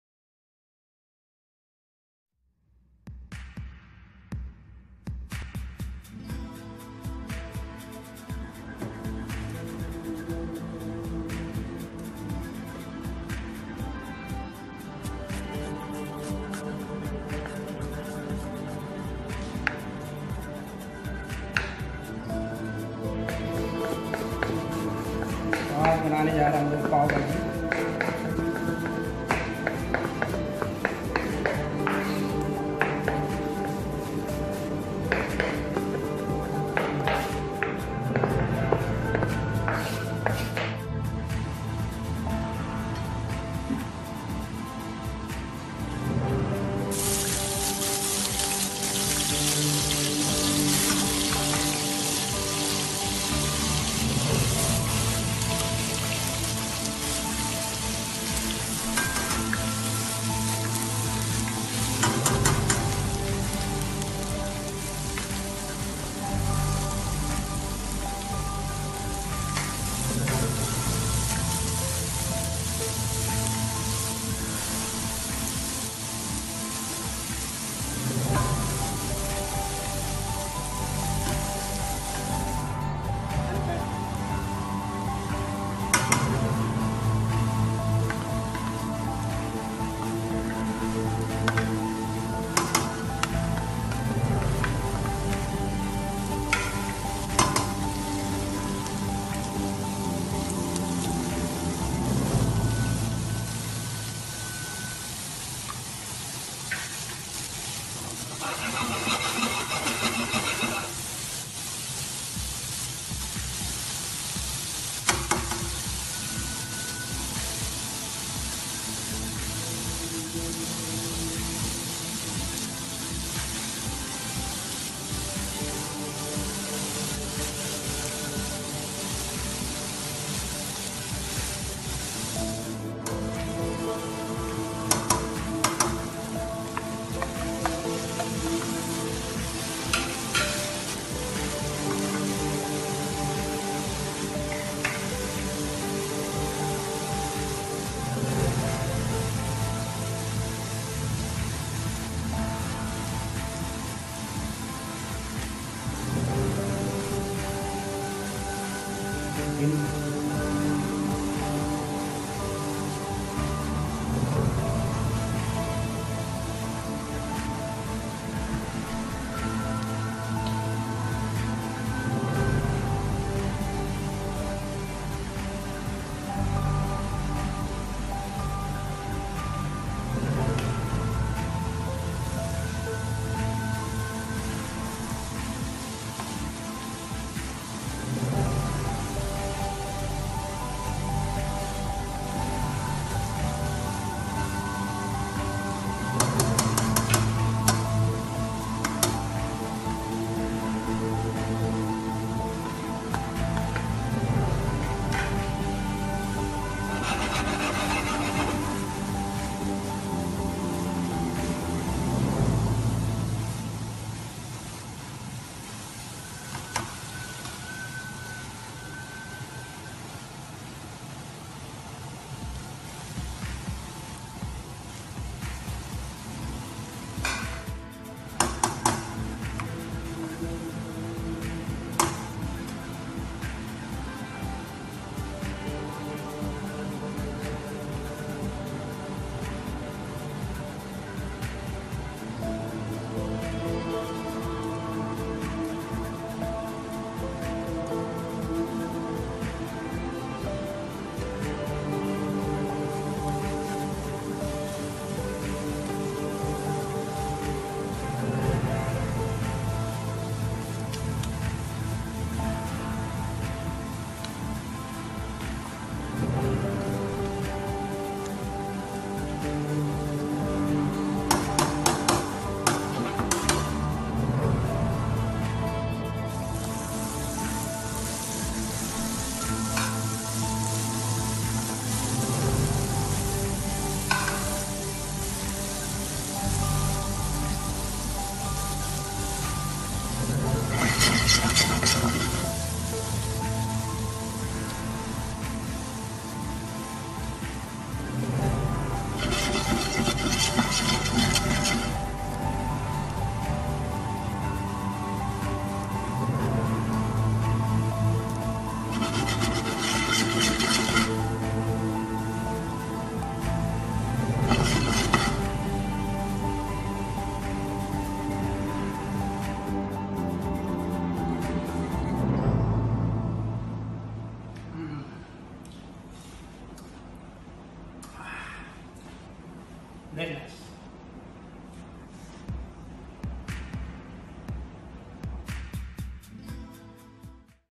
Mumbai street style pav bhaji famous pav bhaji in India pav bhaji recipe quick pav bhaji
1  how to make pav bhaji easy and quick
  2 learn how to make pav bhaji recipe delicious Mumbai street  food i
yummy pav bhaji
INGREDIENTS
cauliflower
green chili
tomato chopped
capsicum chopped
coriander chopped
boiled potato
butter
onion chopped
lemon
salt
red chili powder
bhaji masala